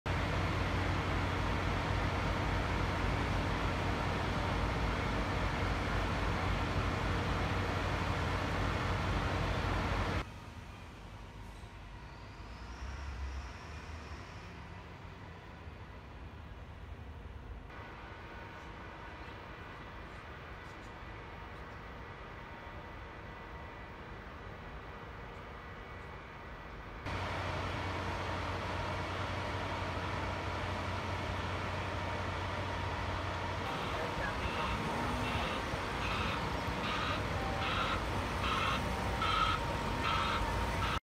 Firemen at work after blaze rips through Glasgow Art School
Fire and emergency services work after a fire rips through one of the world's top art schools in the Scottish city of Glasgow, as the historic building was undergoing major restoration work following another blaze four years ago. IMAGES